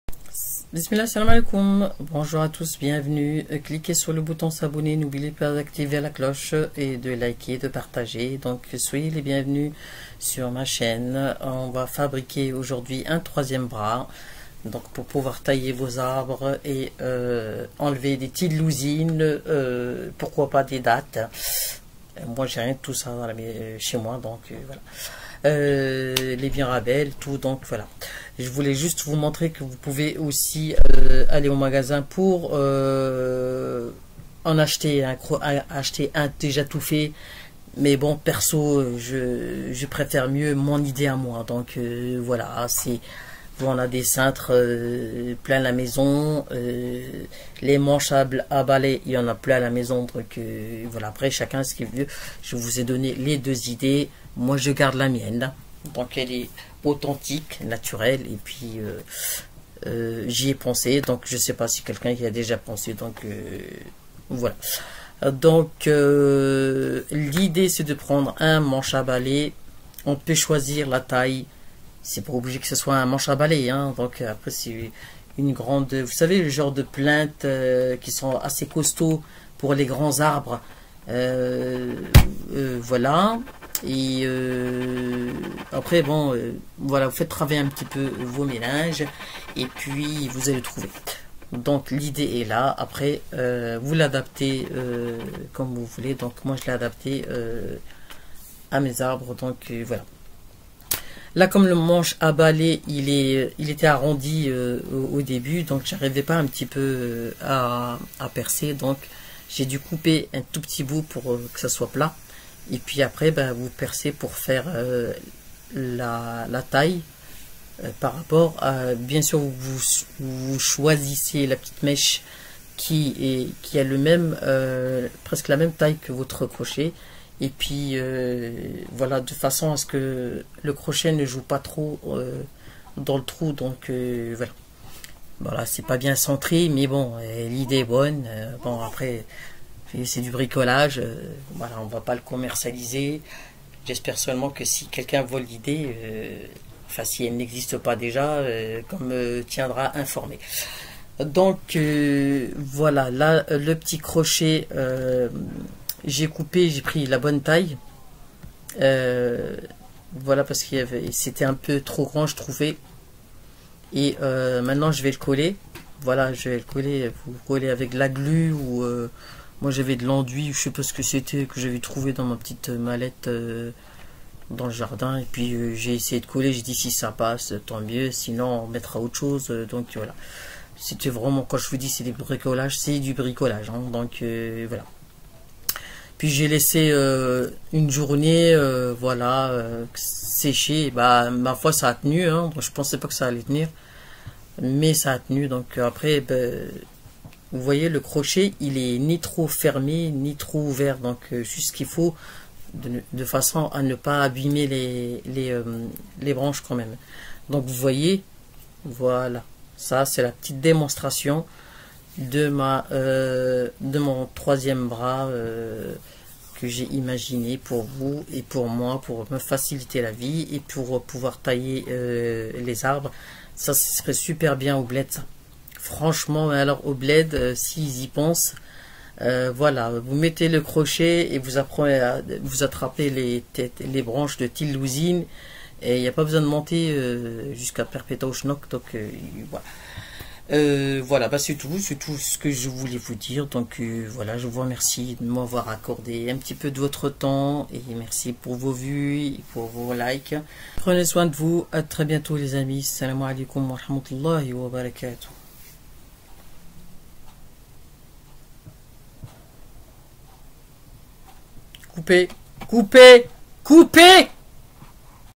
Bricole Ta Propre Perche Télescopique/ اجعل حديقتك قطبا تلسكوبيا /Make your garden telescopic pole
Avec un ceintre et un manche a balai , bricoler votre propre perche télescopique
Je fais tous avec depuis ! je tire sans monter l'échelle!
Je taille mes arbres avec , tous ce que je peux pas s attraper ,
je l'attrape avec mon 3èeme bras !
MALIN ! NON ?